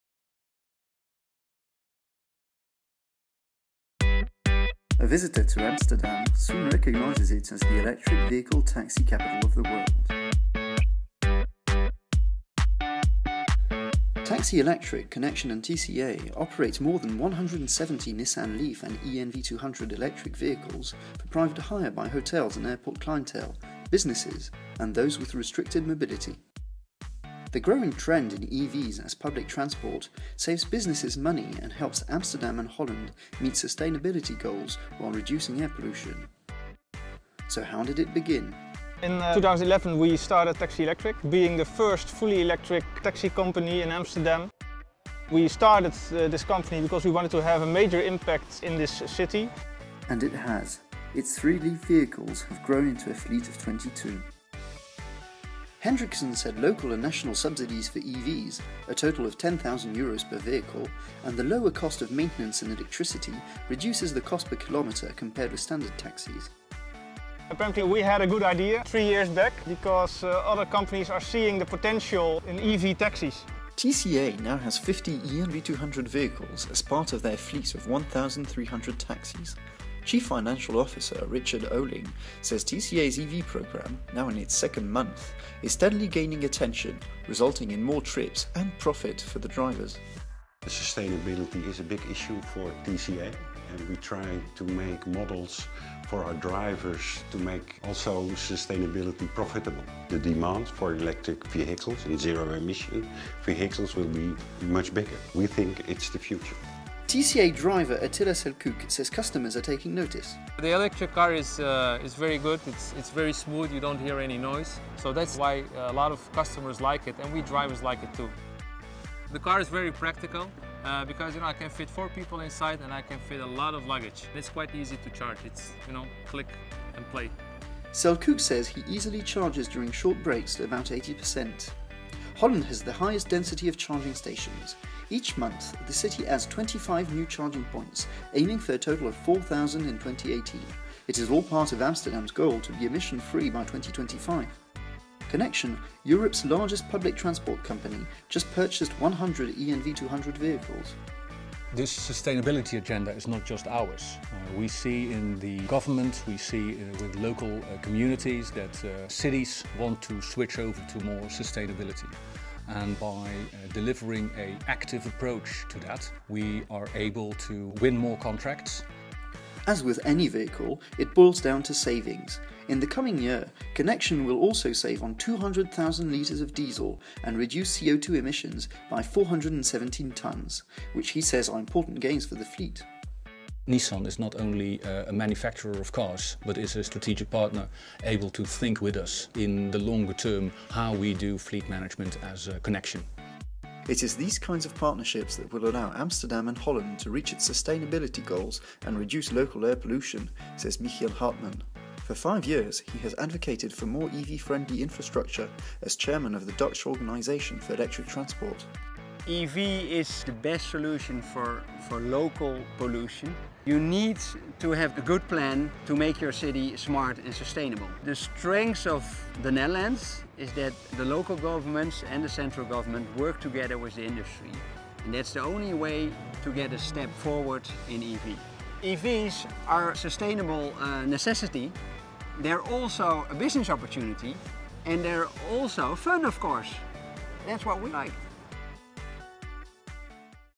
Hail the Electric: Amsterdam's EV Taxis
Amsterdam is the electric vehicle taxi capital of the world, as more taxi companies operate Nissan LEAF and e-NV200 vehicles.